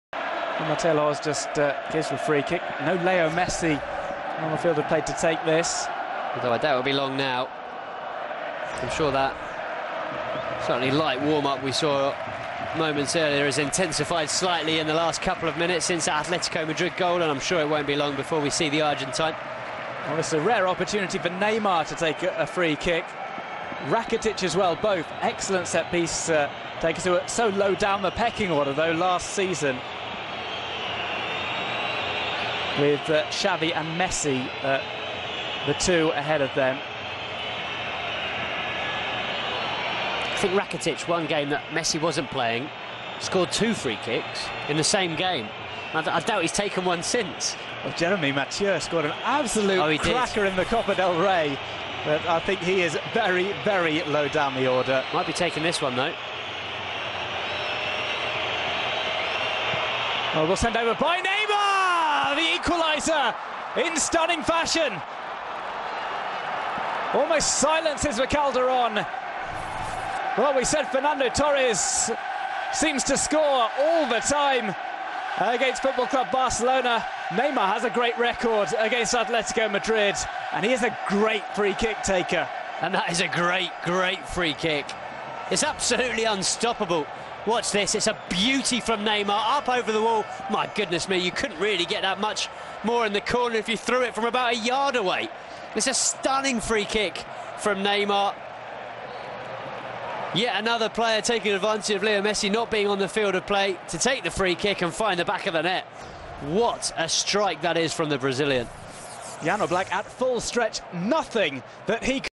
Neymar Amazing Free Kick Goal vs Atletico Madrid HD
12/09/15 Neymar's free kick against Atletico Madrid at the start of the 2015/16 season